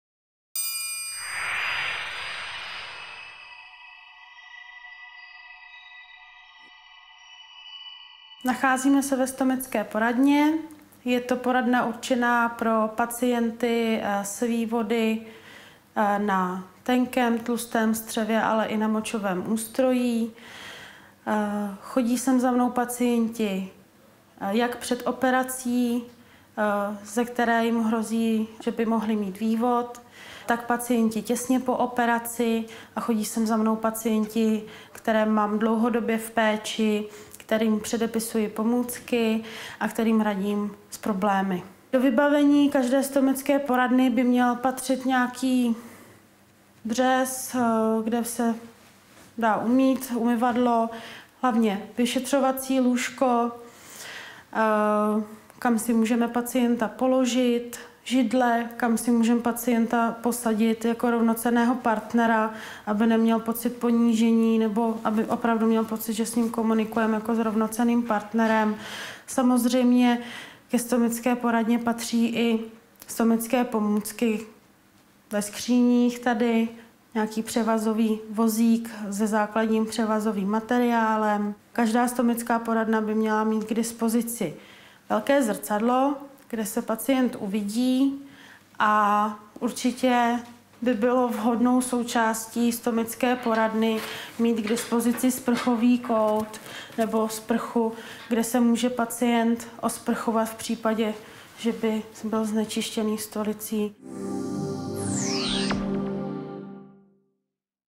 Stomická poradna
Toto video spolu s 51 dalšími je součástí edukativního DVD Průvodce onemocněním kolorektálním karcinomem. Celé DVD je ke stažení na . Cílem projektu je projít náročným obdobím po sdělení diagnózy zhoubného nádoru. Filmy poskytnou pacientovi možnost stát se partnerem svému ošetřujícímu lékaři v rozhodování o další diagnostice a léčbě nemoci.